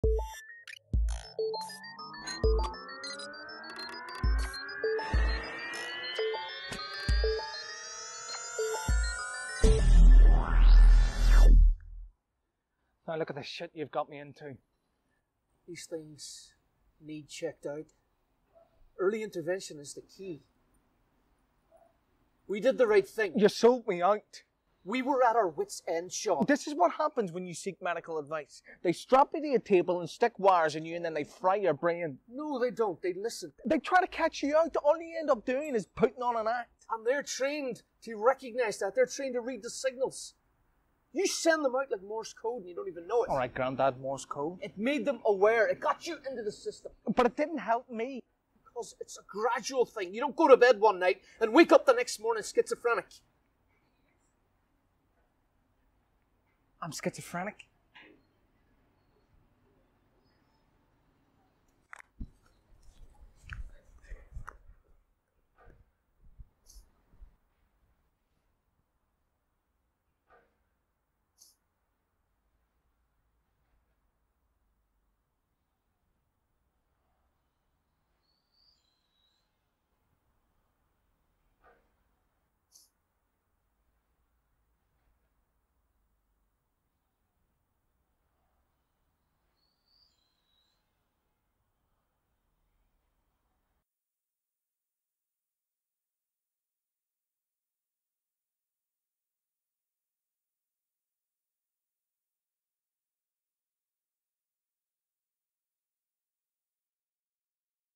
Nutcase by Colin Bateman | Playhouse Derry
“Here's a fact. More people are murdered by vegetarians than schizophrenics. But you wouldn't think it.”

We are welcoming back our audiences!

And we're delighted that our first live audiences in 15 months will be for the world premiere of a new play by best-selling novelist Colin Bateman,

Directed by BAFTA award winning director Kenny Glenaan, Nutcase is a blackly comic but unstinting journey through mental illness.

Running from 24 to 29 August Nutcase will be performed to audiences at The Playhouse, broadcast live and available to stream afterwards.

Kindly funded by Garfield Weston Foundation